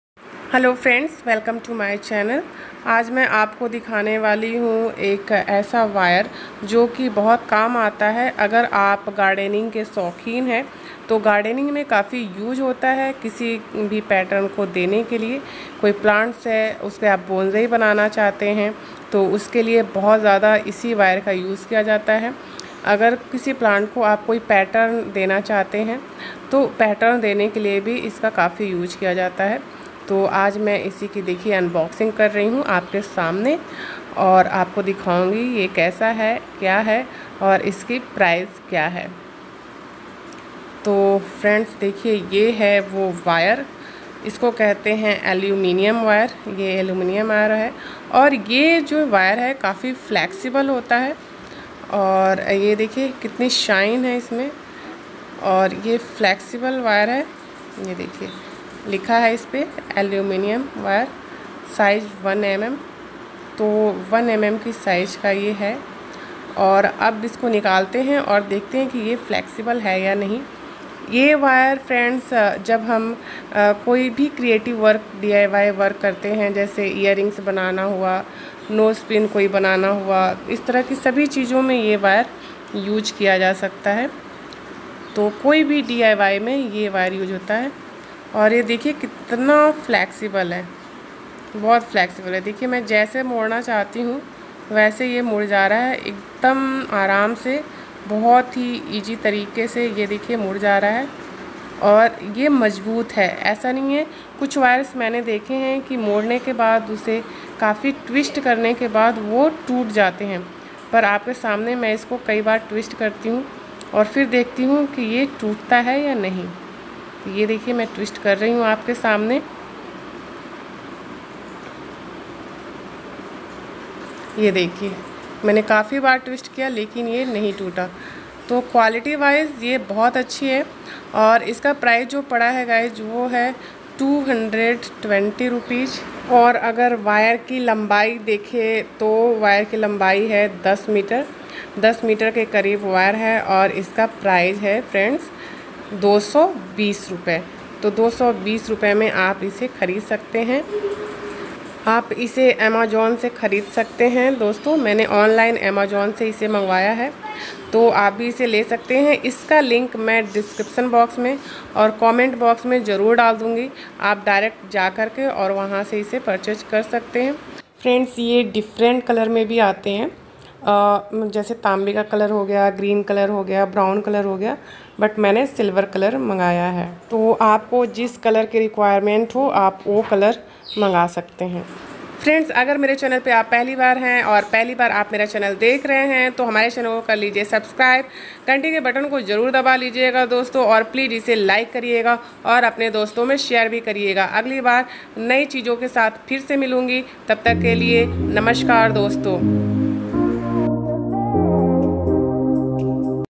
Unboxing/Review Of DIY Aluminium Wire || Buy Online,Review in hindi | Make Bonsai Plant
Unboxing/Review Of DIY Aluminium Wire ||  Unboxing & Review in hindi